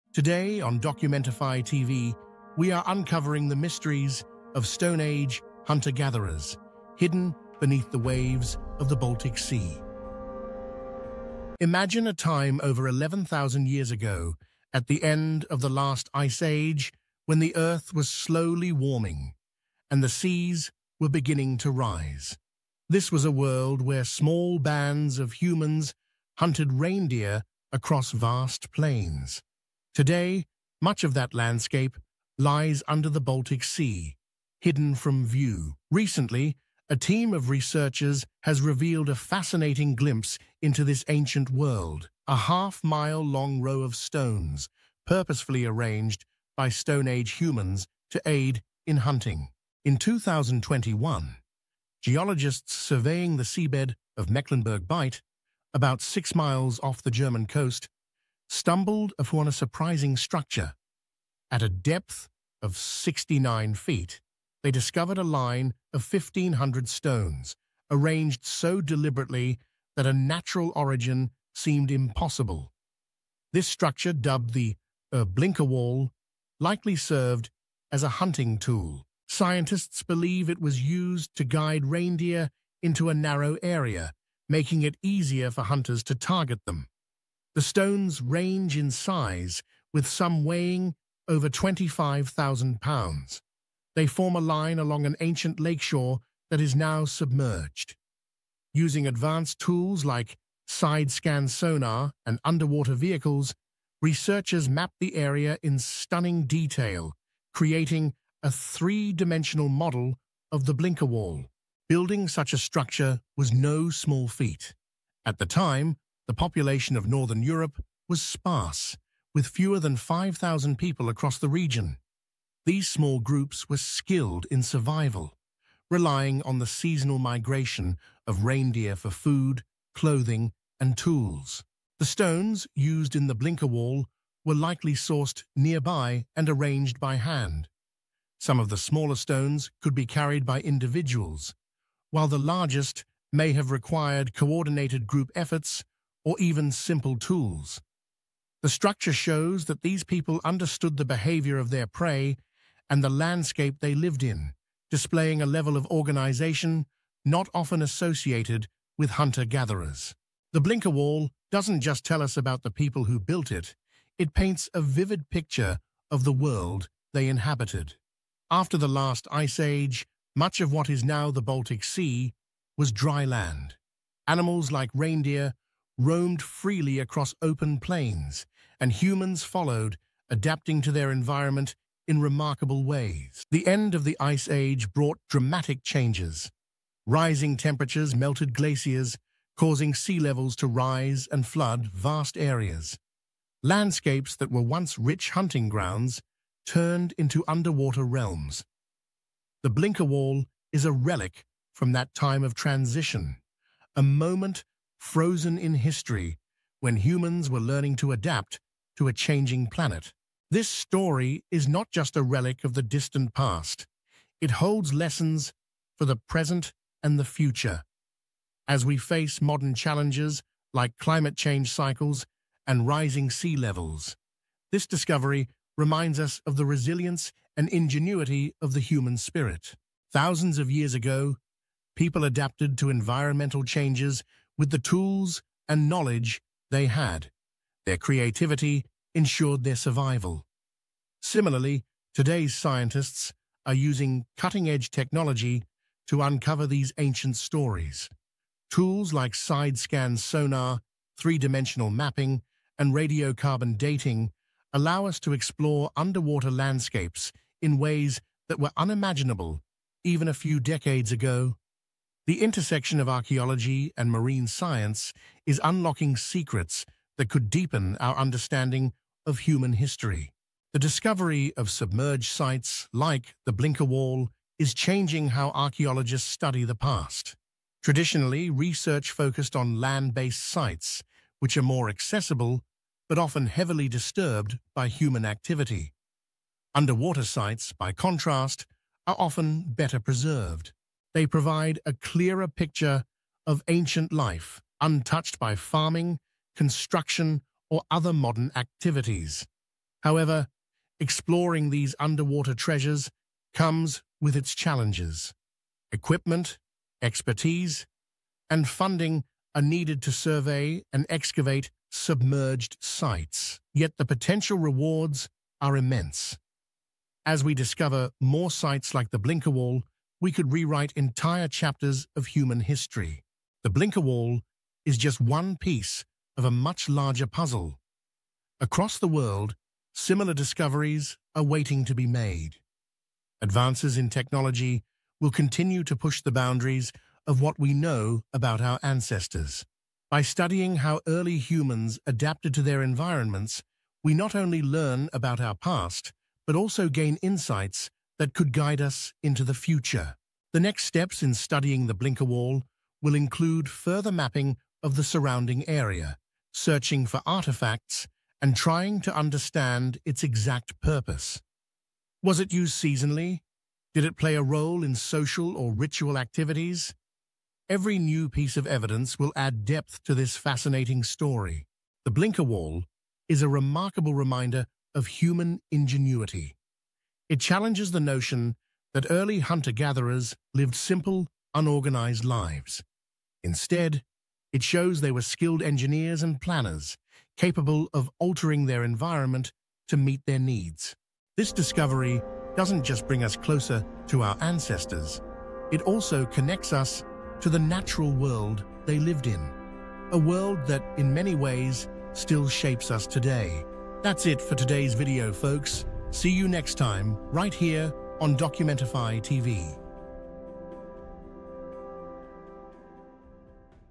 Stone Age Activity Found in the Baltic Sea
Join us in today’s episode as we uncover an 11,000-year-old mystery hidden beneath the Baltic Sea! 🌊
In this video, we explore the recently discovered "Blinkerwall," an underwater Stone Age hunting structure built by prehistoric humans to guide and hunt reindeer.

This half mile-long wall, submerged 70 feet below the waves, reveals the ingenuity and survival strategies of ancient hunter-gatherers.

Learn how scientists uncovered this incredible site using cutting-edge technology like sonar mapping and underwater vehicles and discover what this means for our understanding of human history. Don’t miss this journey into the past! 🕵️‍♂️

📌 Key Topics Covered:
• What is the Blinkerwall?
• How and why Stone Age humans built this structure
• The tools and methods scientists used to uncover it
• The role of climate change and rising seas in submerging ancient landscapes

Sources/Image Credits

Credit: Proceedings of the National Academy of Sciences (2024). DOI: 10.1073/pnas.2312008121